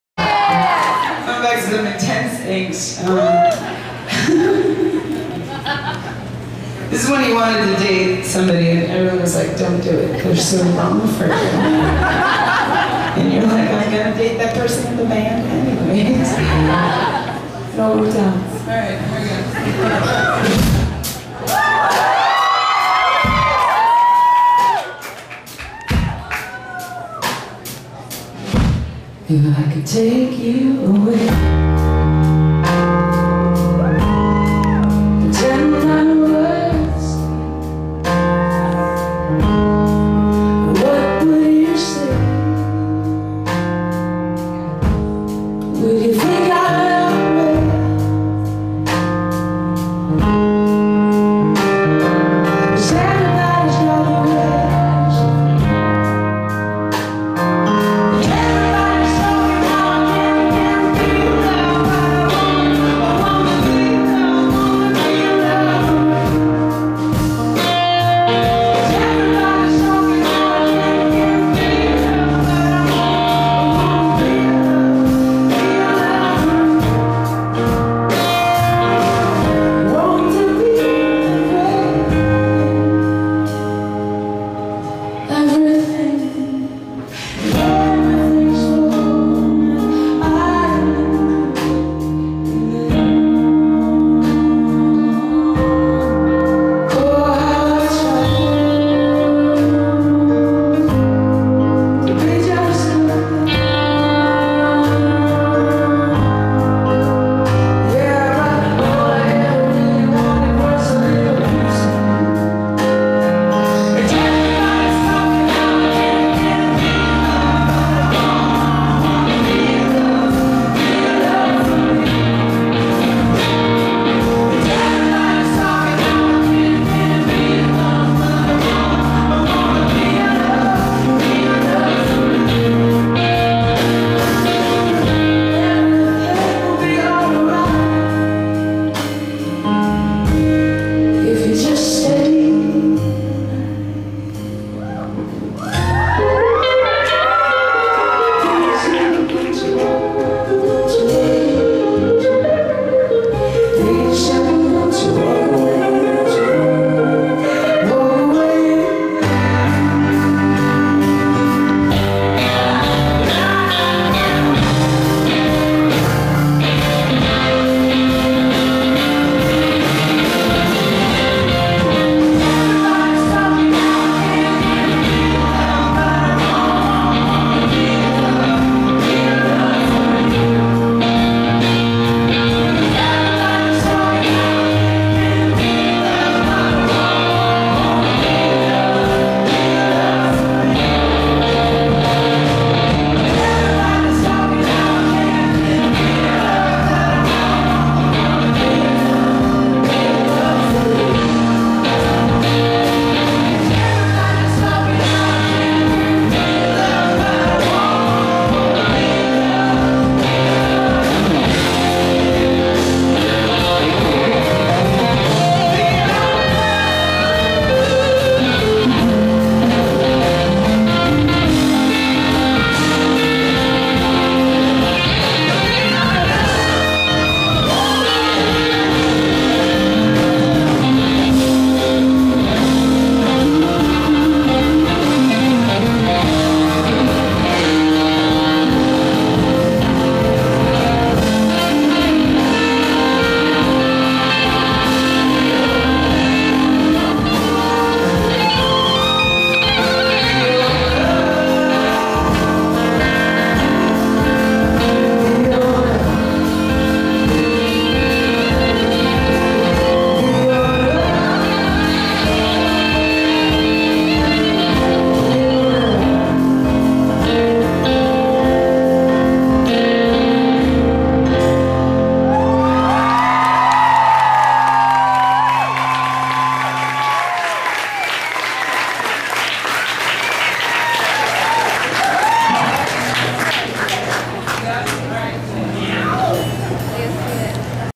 Rachael Yamagata (Live)
Rachael Yamagata performing  live at The Troubadour, Hollywood CA, USA 10/27/14.